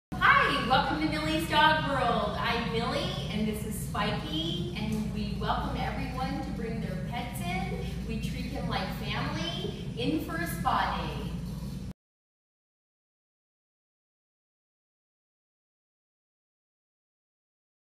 Millies Dog World
Shops on 19ave that are affected by the light rail construction.

Dog and cat grooming
Pet salon

A free iphone video to help them survive the construction.